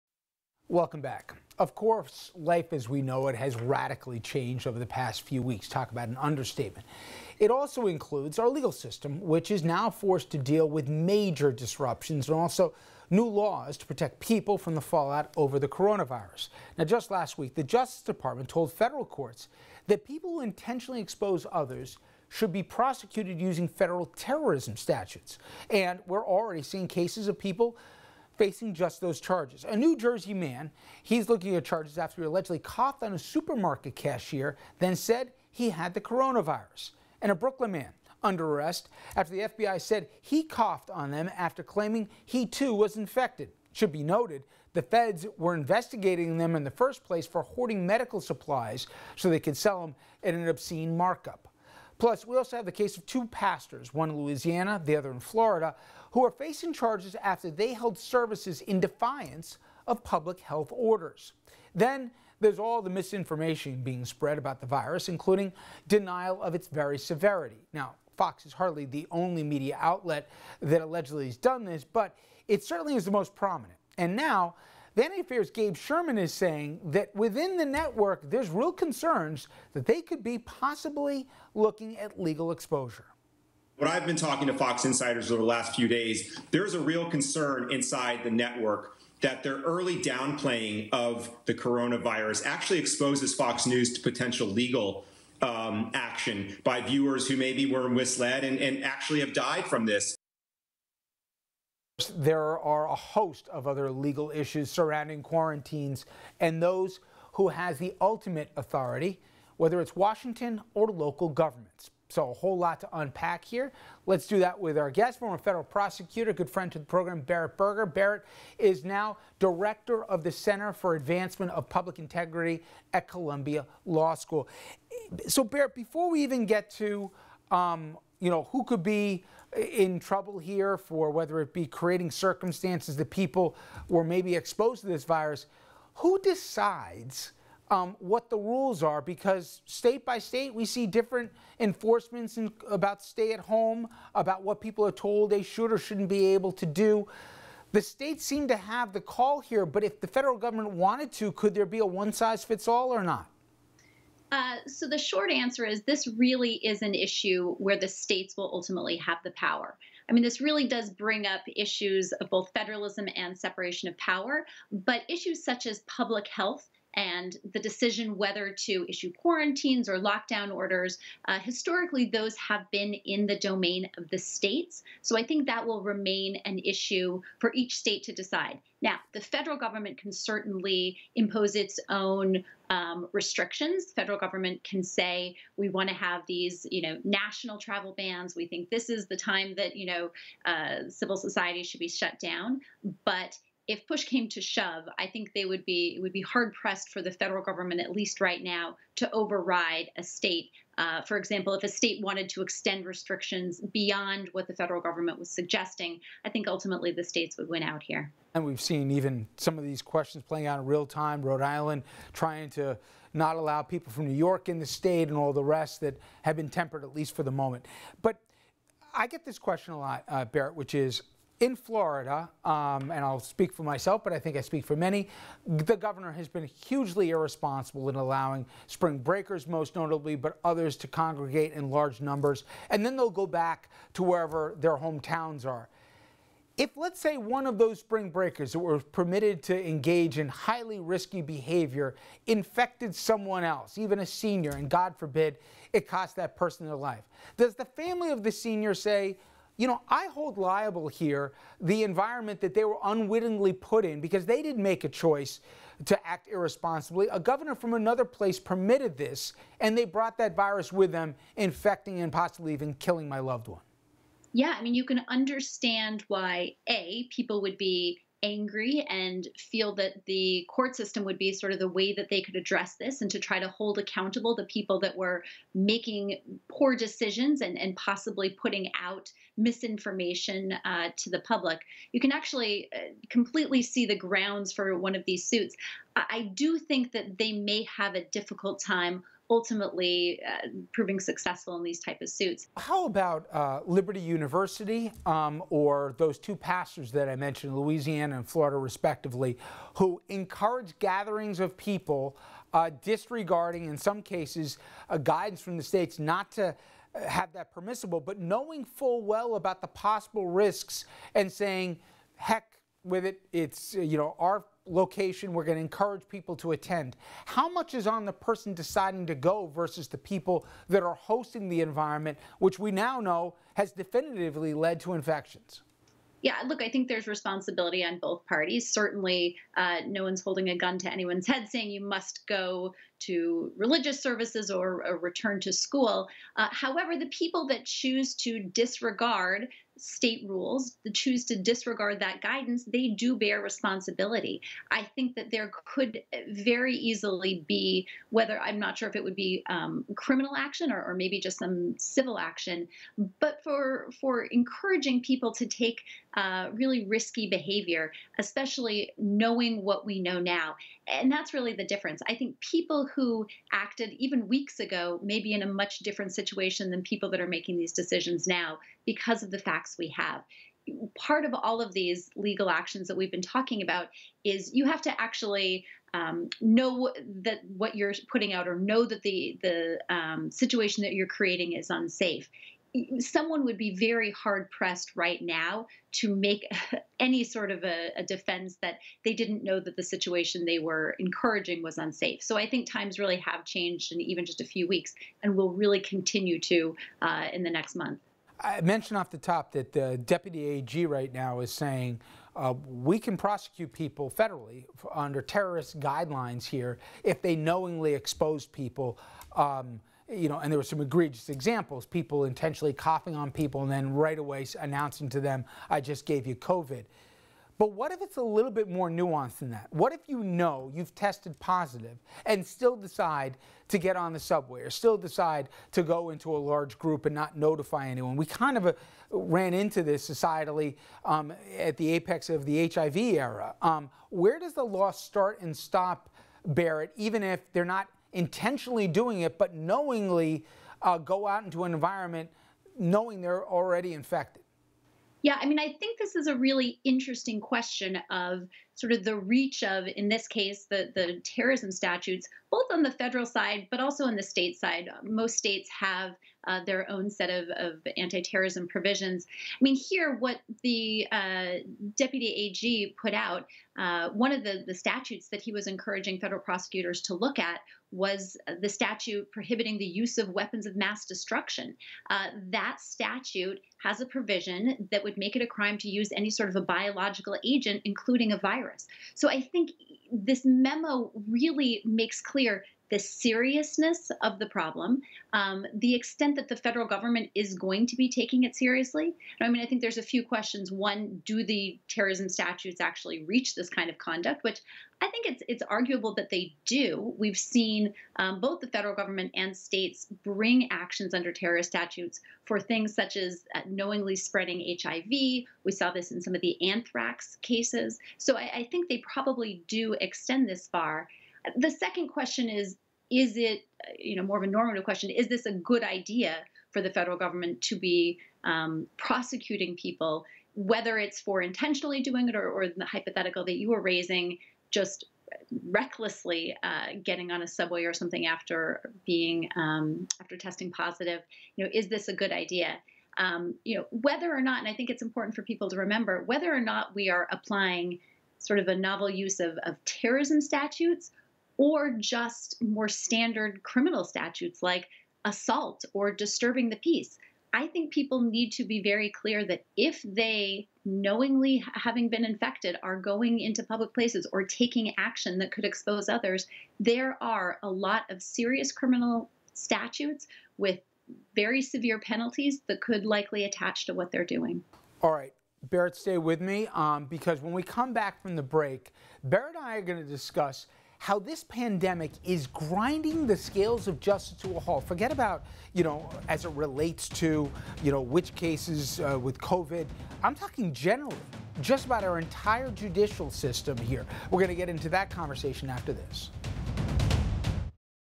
How the Legal System is Dealing with the Pandemic
Richard French talks one-on-one with former federal prosecutor Berit Berger.  How things prosecuted are changing during this pandemic.